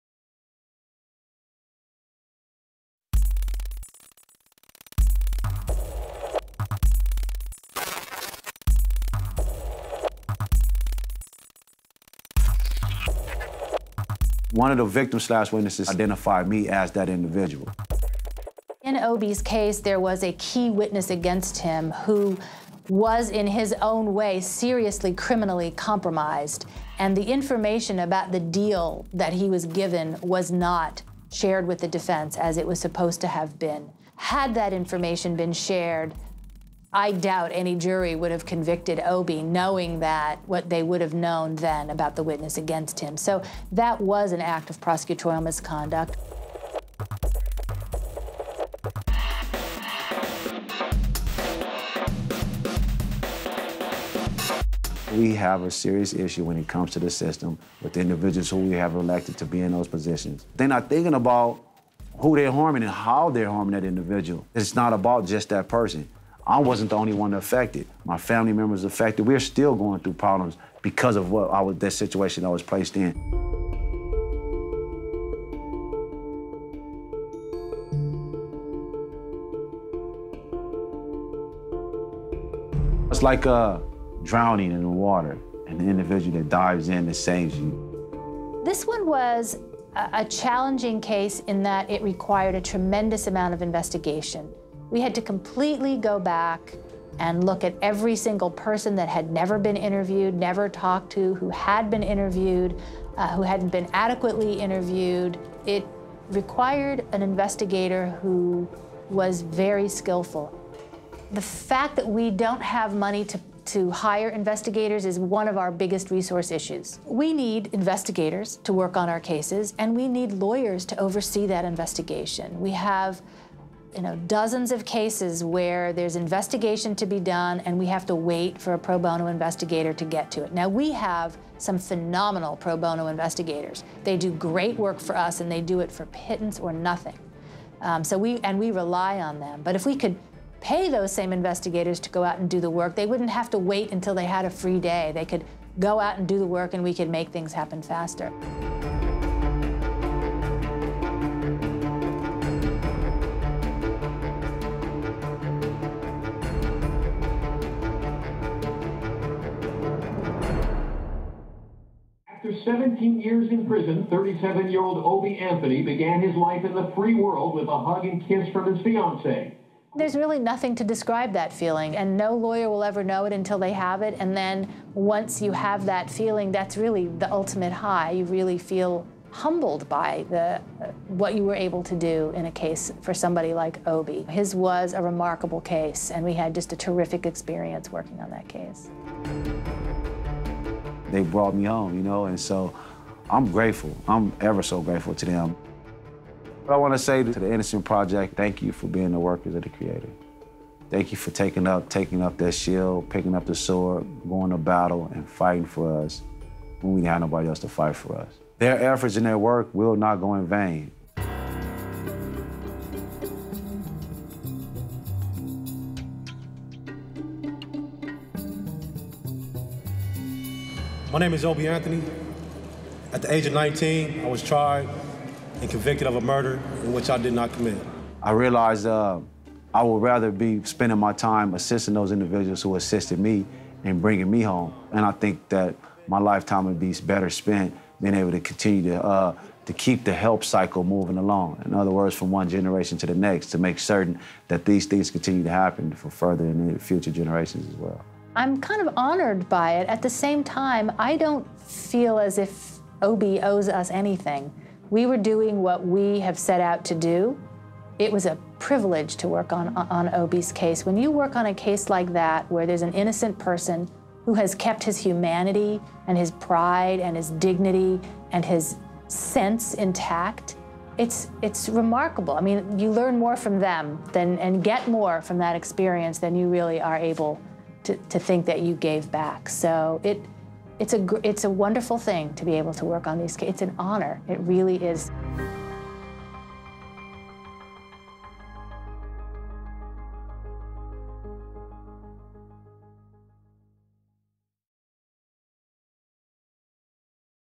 Northern California Innocence Project: Obie Anthony's Story
The wrongful conviction and exoneration of NCIP client Obie Anthony.